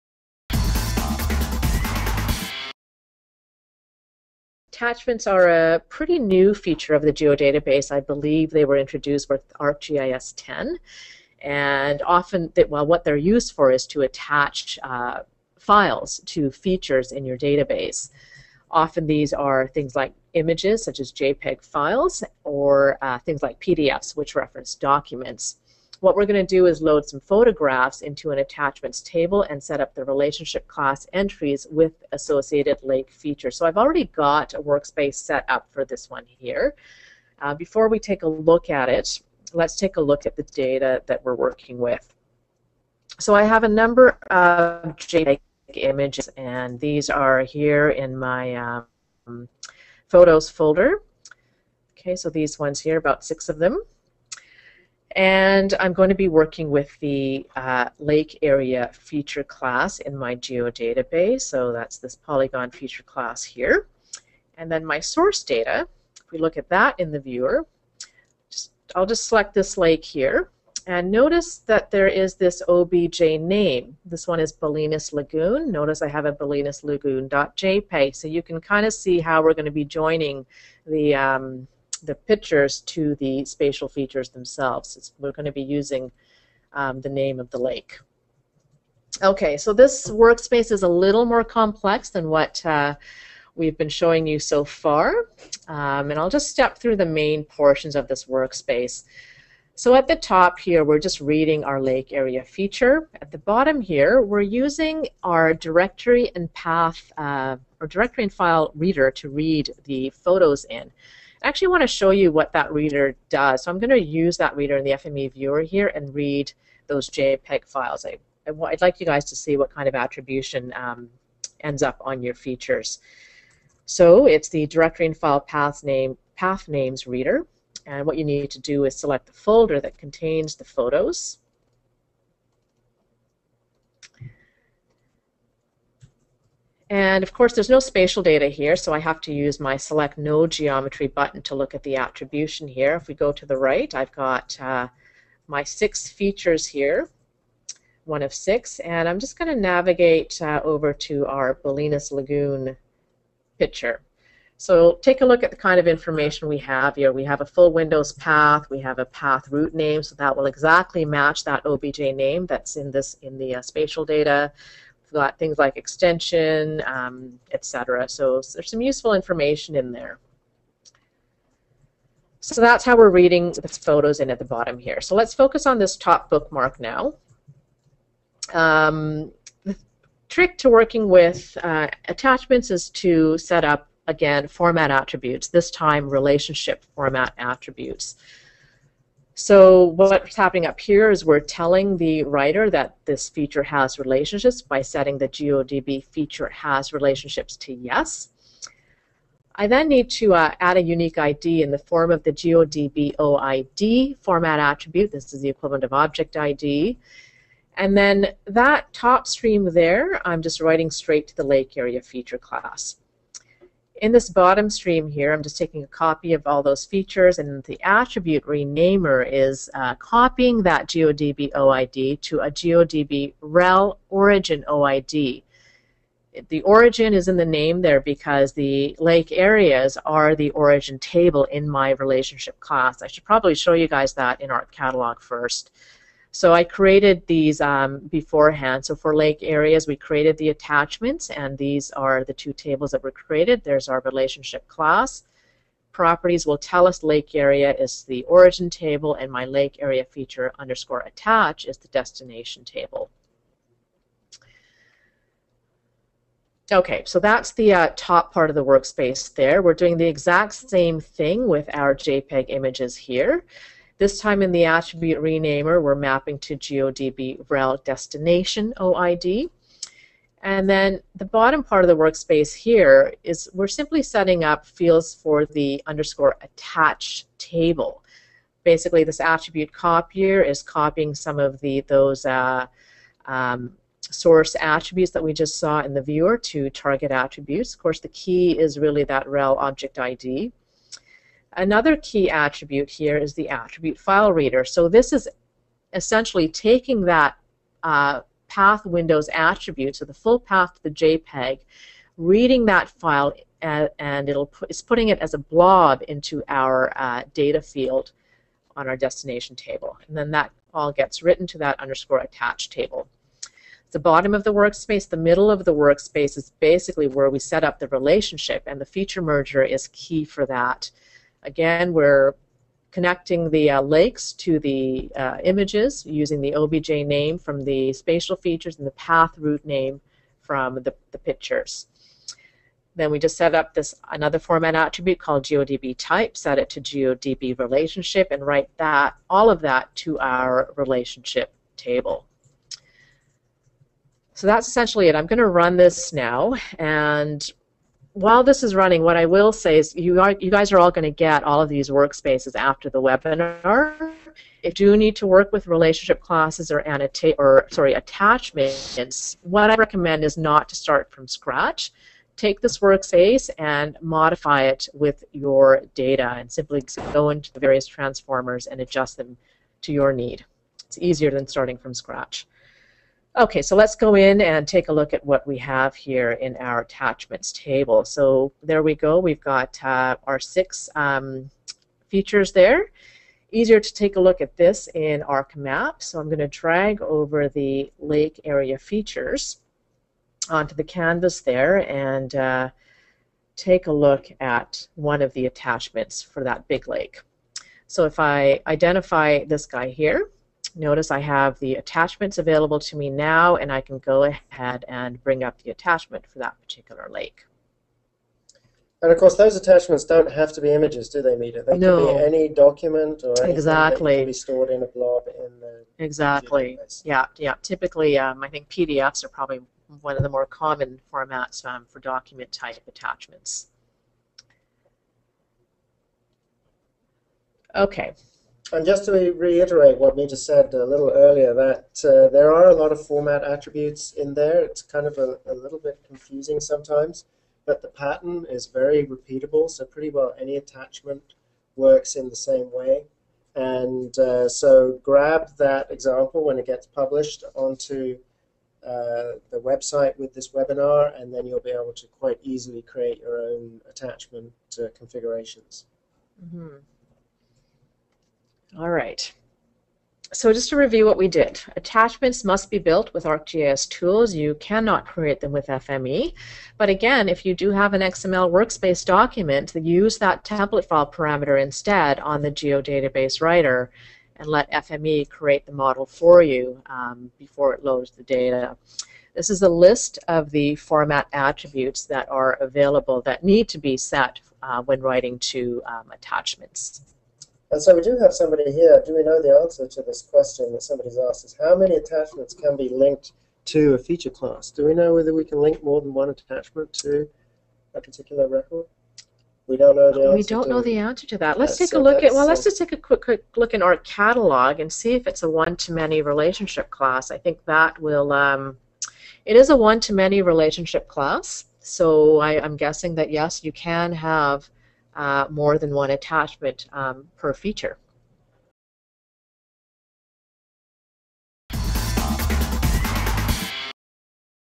Attaching files to your geodatabase
In this demo, you'll see how to add attachments to your database using FME Workbench. Attachments are used to attach files to features in your database, like JPEG images or PDF documents. We'll create a workspace to load JPEG images into an attachment table, and set up the relationship class entries with the associated features. Our workspace reads S-57 (ENC) Hydrographic Data and writes to an Esri File Geodatabase, and uses FME transformers to set up the necessary format attributes.

Transformers used: Counter, AttributeRenamer, AttributeCreator, AttributeCopier, AttributeFileReader, FeatureMerger, AttributeRemover